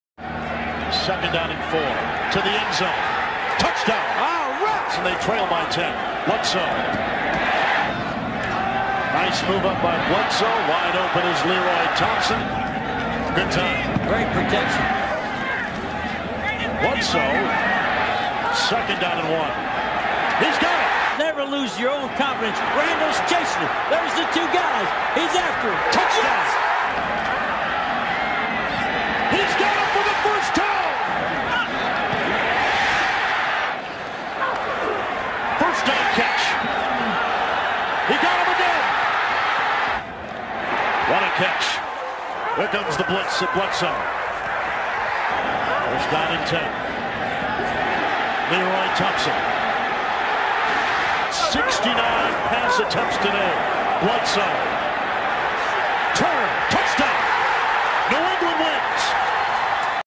Most Completions In A NFL Game (1994)
Drew Bledsoe set the record for pass attempts (70) and completions (45) in a single game against the Minnesota Vikings on Nov. 13, 1994